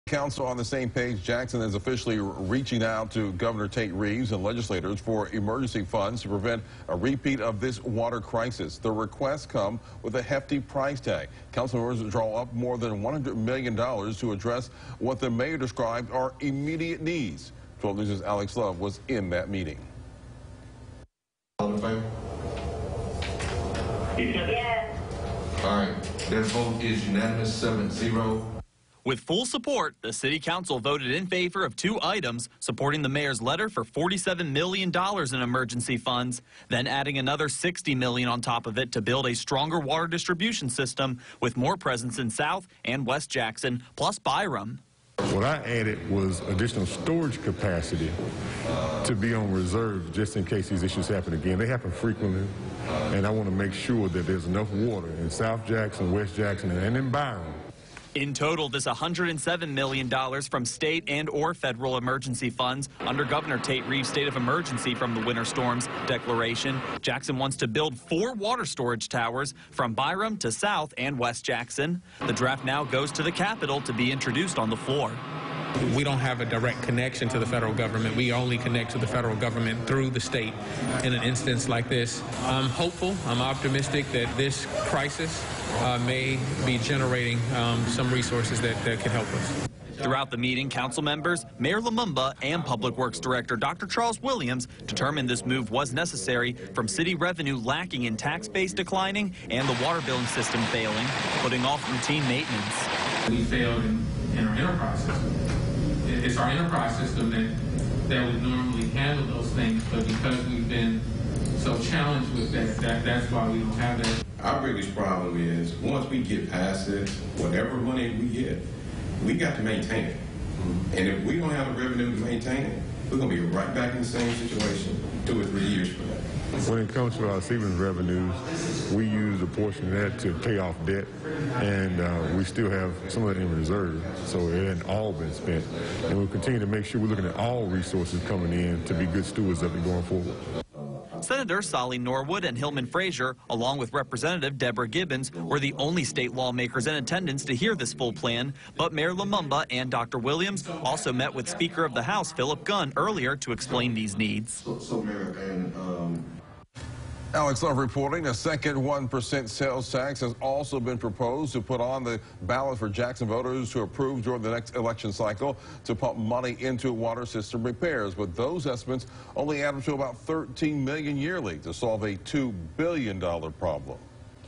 Jackson leaders to request $60 million to fund water storage towers
With City Councilman De'Keither Stamps of Ward six now the voice for District 66 in the Mississippi House of Representatives. He called this meeting with a few other legislators drawing up more than $100 million to address what the mayor described are "immediate needs."